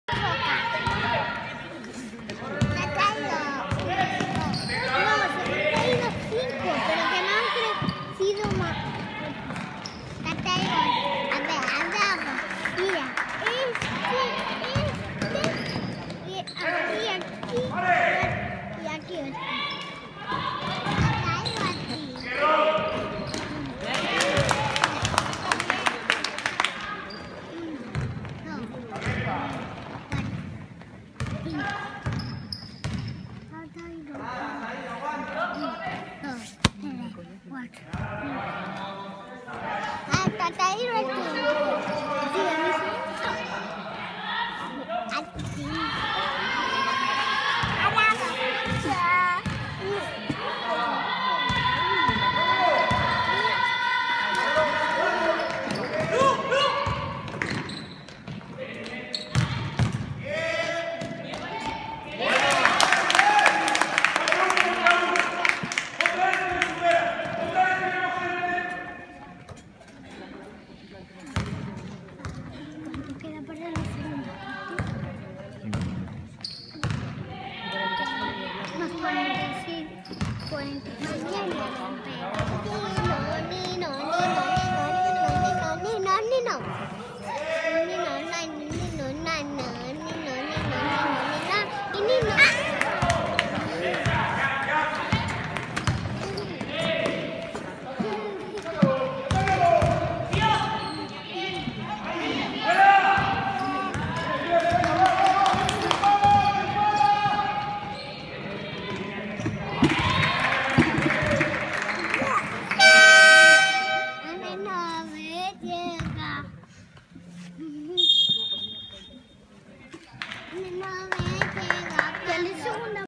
VILLANUEVA DE LA CAÑADA-OLÍMPICO 64 SANTA GEMA.; 1ª Autonómica Femenina 2015-2016 (1/2)
3 de Abril de 2016
Pvo.Mpal.Santiago Apóstol
Villanueva de la Cañada